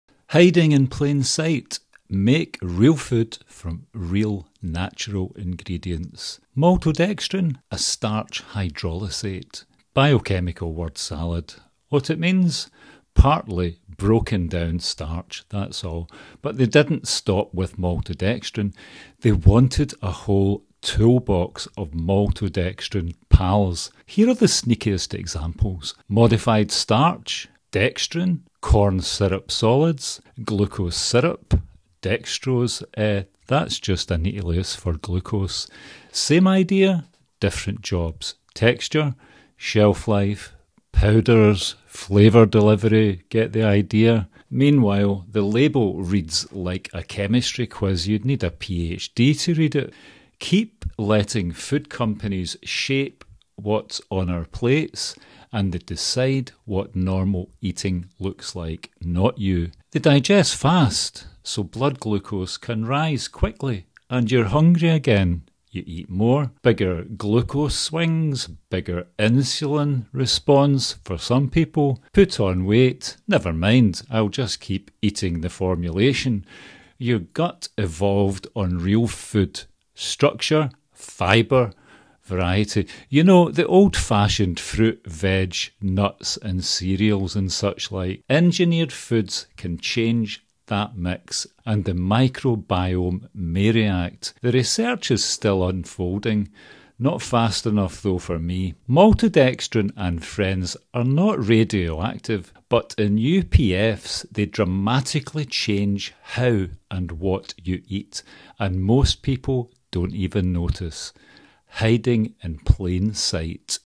hiding in plain sight - maltodextrin
HIDING IN PLAIN SIGHT — maltodextrin is everywhere in ultra-processed foods (UPFs), often under “friendly” label language. In this Short I show what maltodextrin really is (a starch hydrolysate), the common label relatives to watch for (modified starch, dextrin, glucose syrup, corn syrup solids, dextrose), and why fast engineered carbs + low fibre can mean a quicker blood sugar hit for some people.
This isn’t fearmongering — it’s label literacy. Real food = more control.